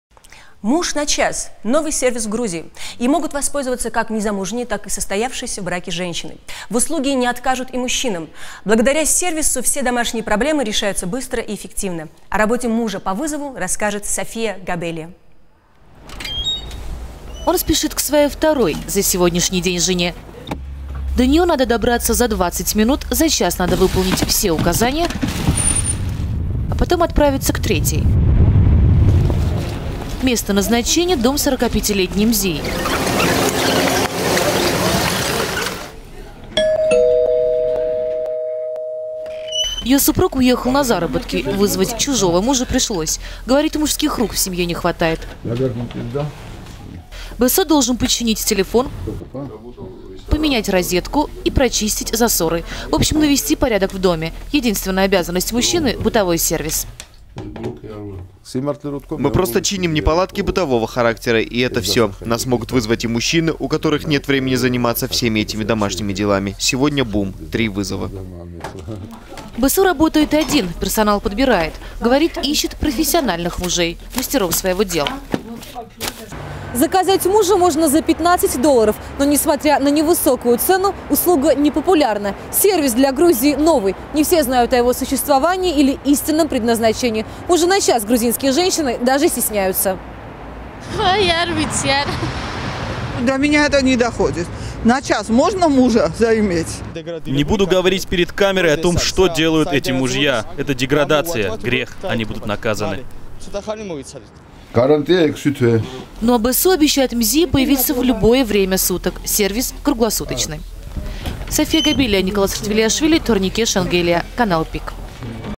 «Муж на час» -- новый сервис в Грузии
Им могут воспользоваться как незамужние, так и состоящие в браке женщины. В услуге не откажут и мужчинам. Благодаря сервису, все домашние проблемы решатся быстро и эффективно. О работе «мужа по вызову» расскажет София Габелия.